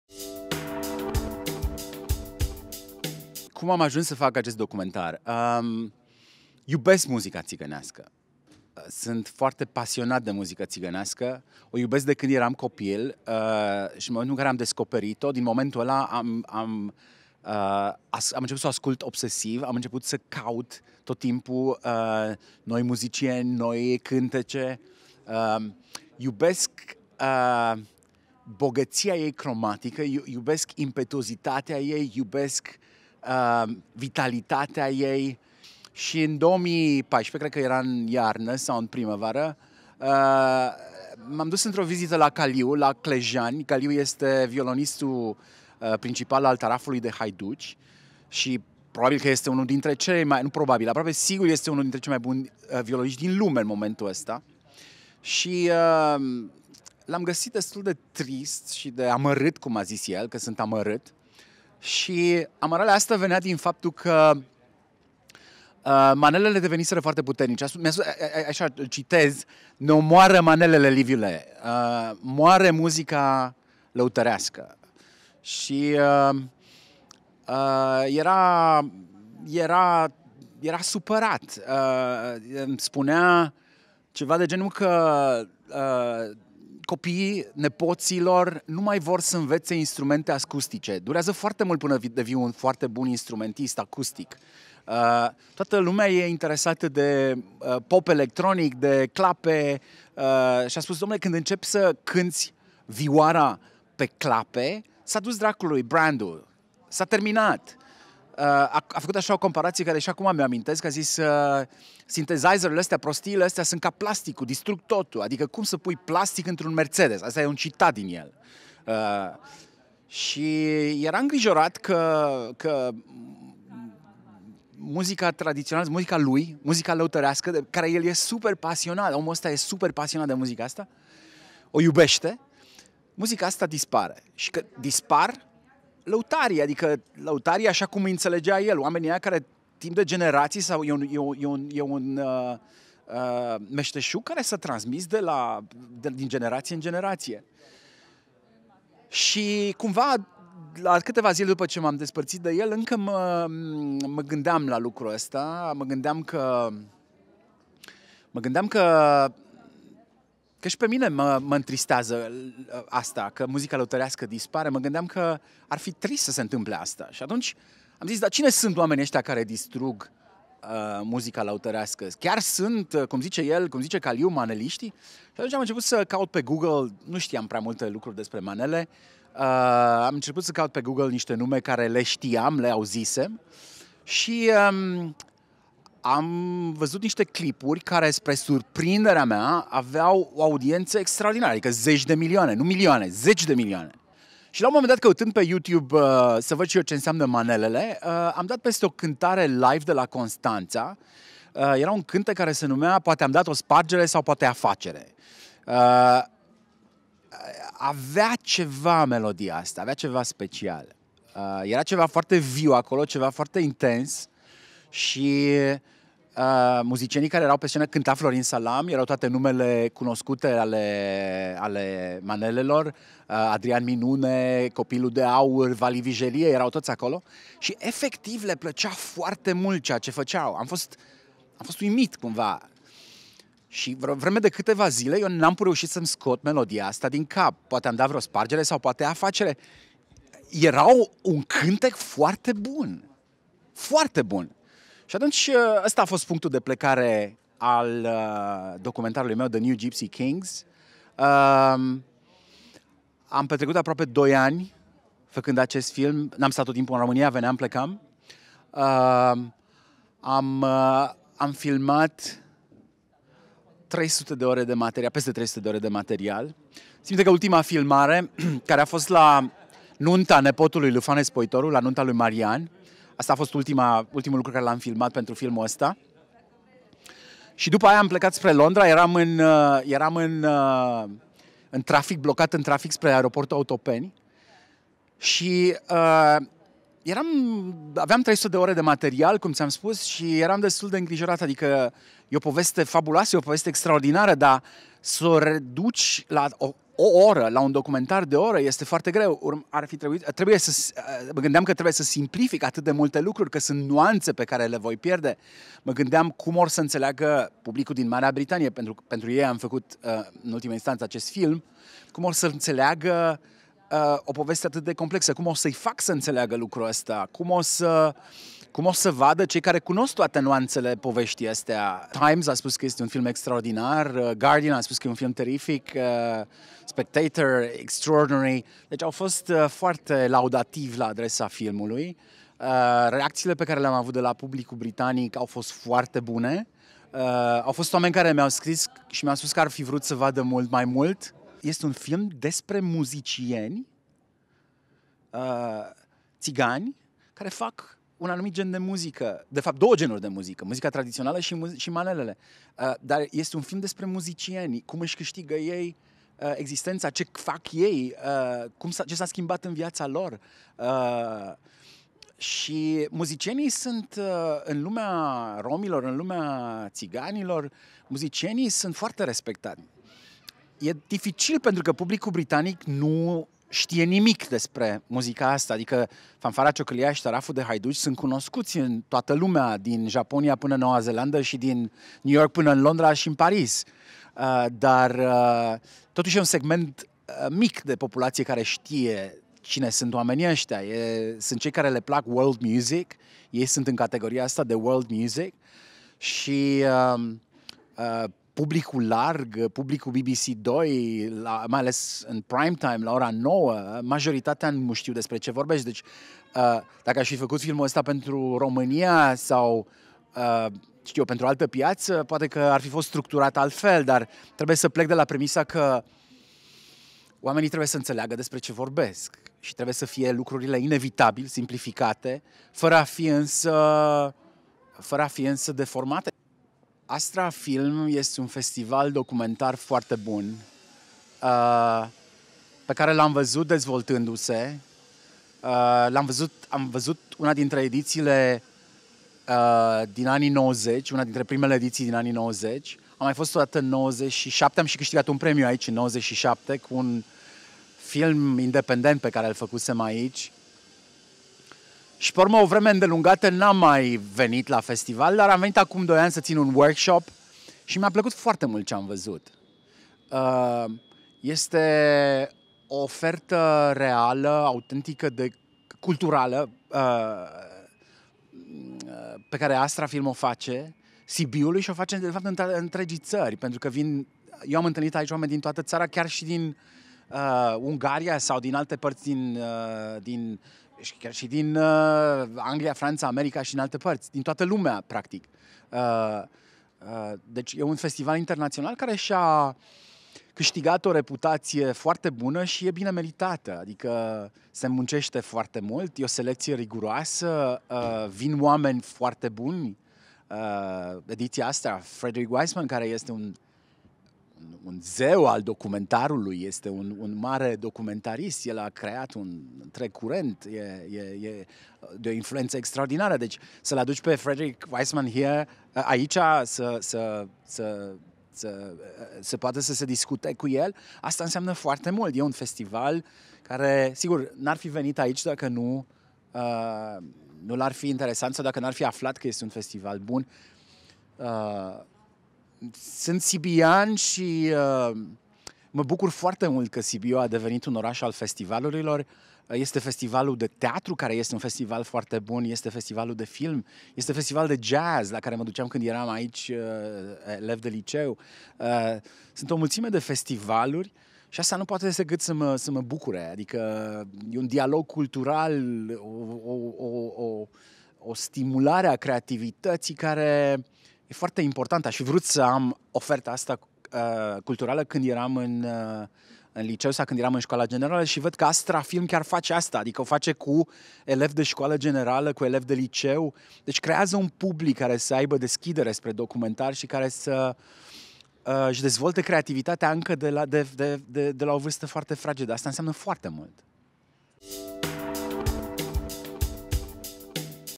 Interviu cu Liviu Tipuriță la AFF 2016
Interviu cu Liviu Tipuriță, regizor si producator la BBC, in cadrul Astra Film Festival 2016